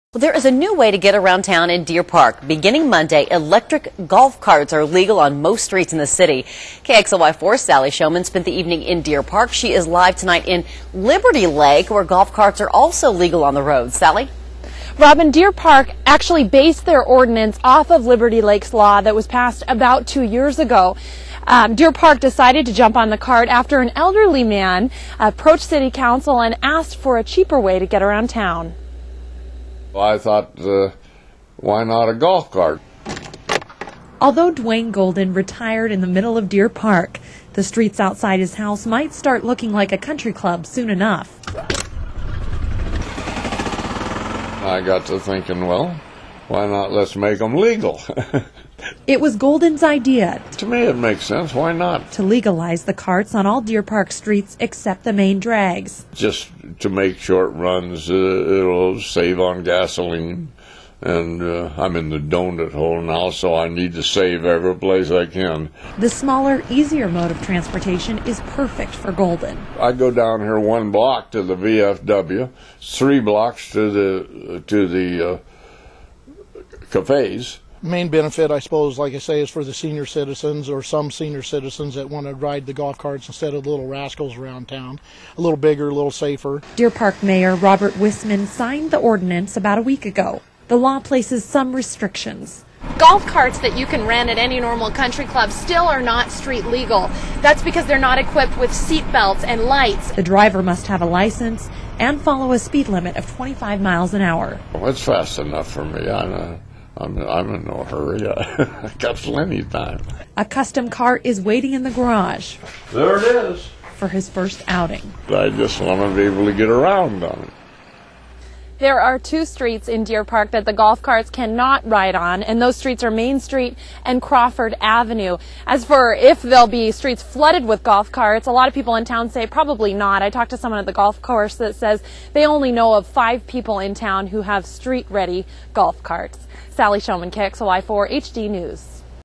Golf carts now legal on Deer Park's streets
A new ordinance allows golf carts to roam the streets of Deer Park.  Restrictions apply, but for one resident, it means more freedom to go where he wants.  KXLY4's Sally Showman reports.